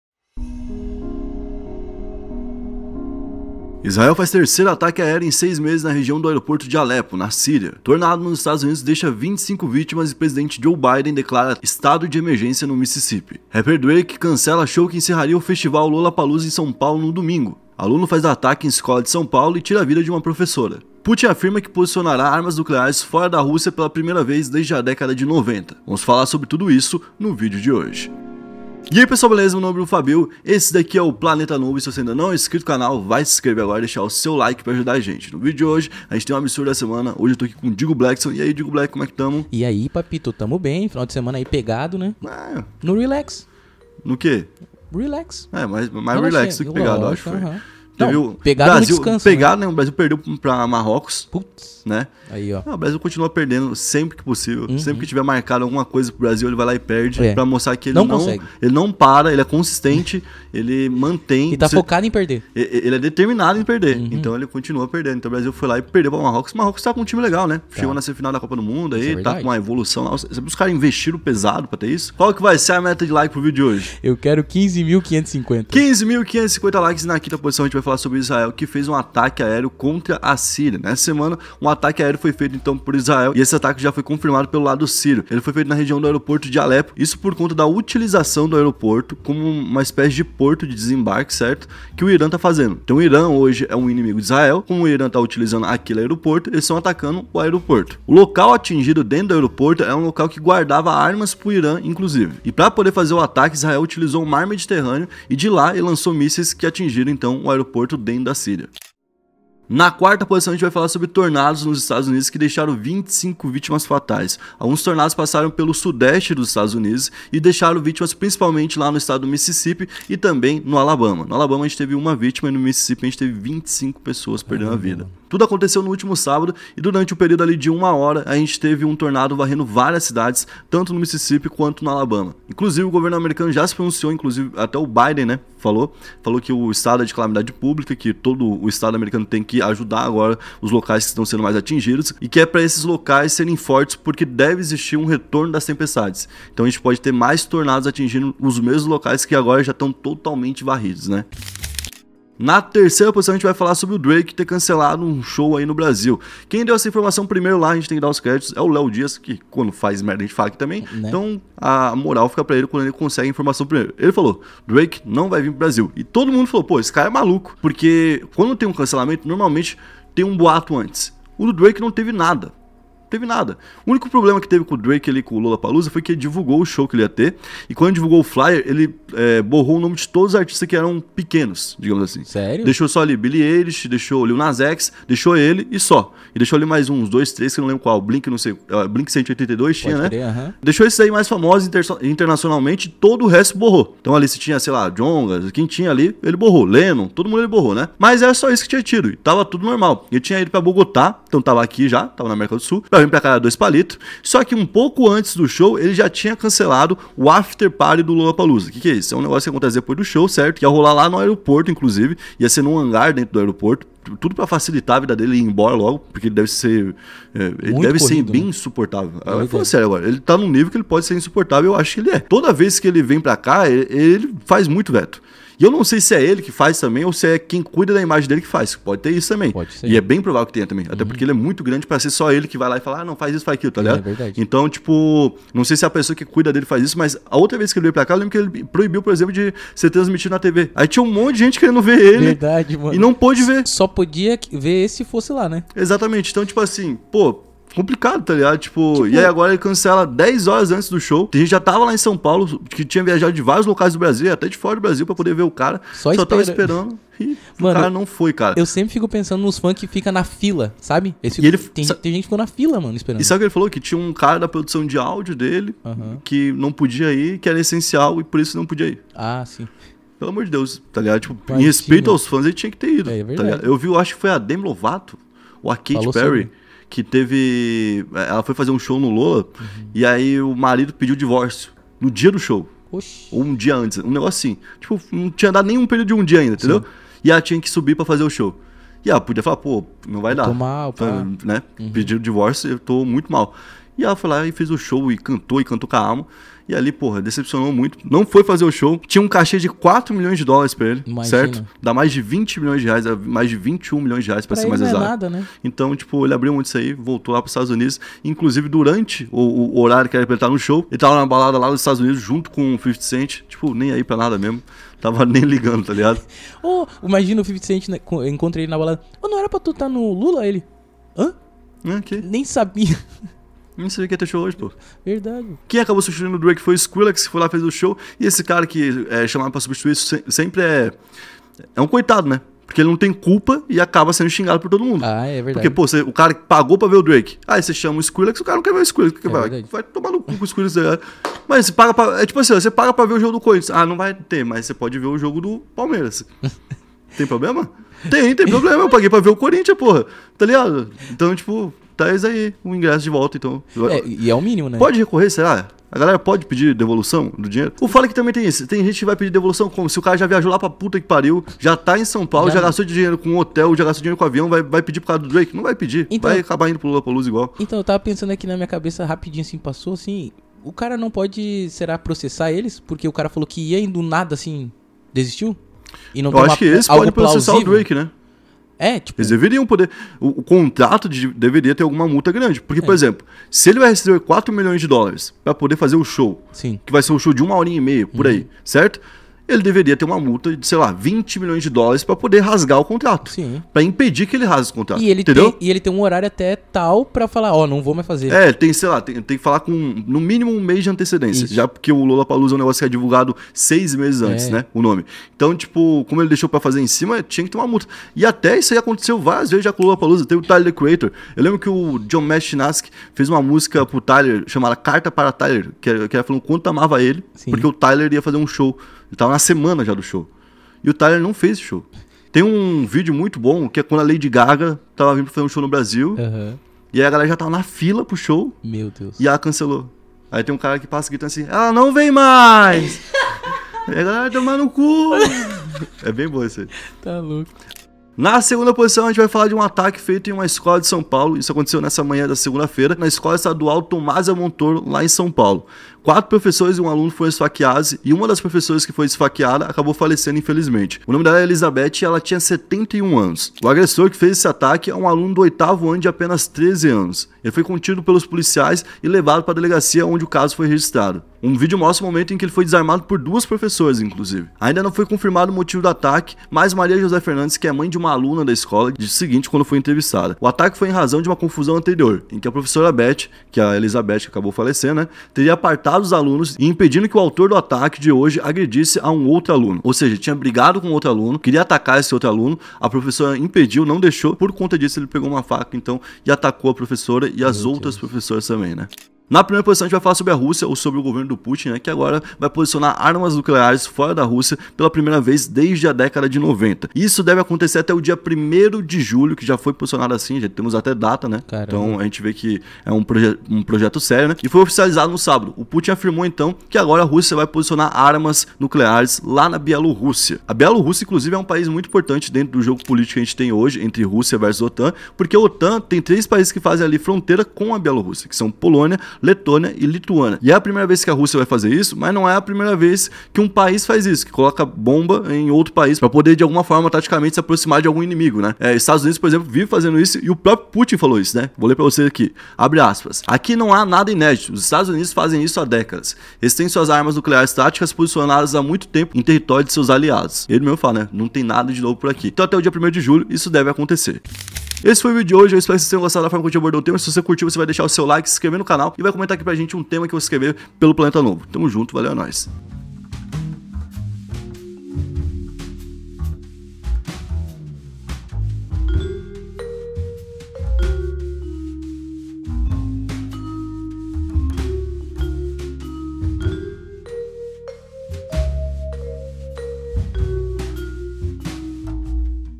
ATAQUE EM ESCOLA DE SP, TORNADO NOS EUA e DRAKE CANCELA SHOW I Absurdos da Semana #283
Absurdos da Semana

00:00 Introdução
01:17 I 5 - Israel faz ataque.
01:50 I 4 - Tornado nos EUA.
02:32 I 3 - Drake cancela show no Brasil.
07:19 I 2 - Ataque em escola de SP.
08:10 I 1 - Rússia vai armar armas nucleares.